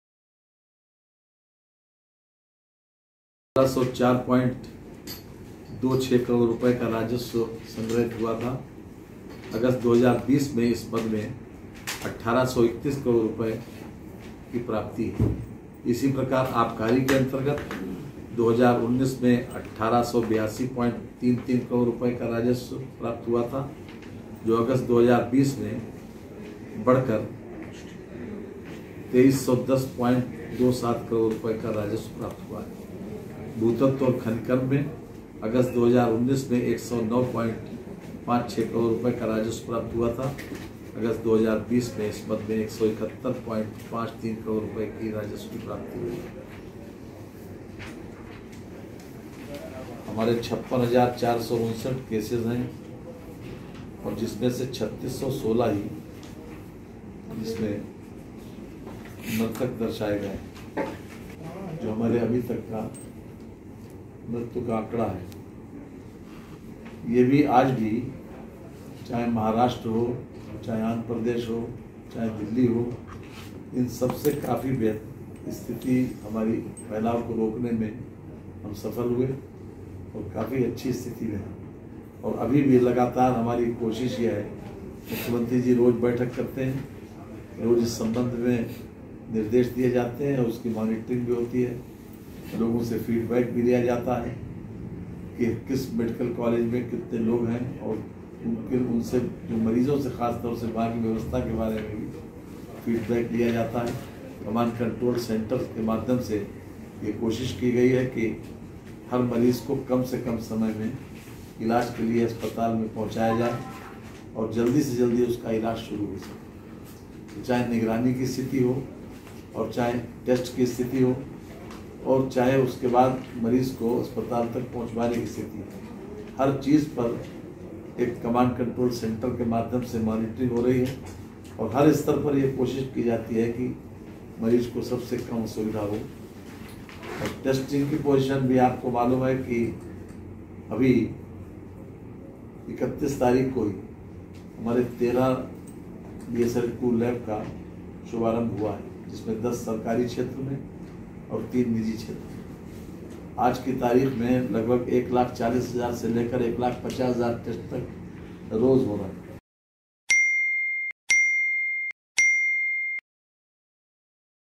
लखनऊ: कोरोना काल में यूपी की अर्थव्यवस्था को लेकर वित्त मंत्री सुरेश खन्ना की प्रेस कांफ्रेंस
यूपी सरकार के दावों के मुताबिक कोरोना संकट के इस दौर में भी राज्य की अर्थव्यवस्था पूरी तरह दुरस्त और सेहतमंद। जीएसटी समेत सरकार के सभी तरह के राजस्व में उछाल दर्ज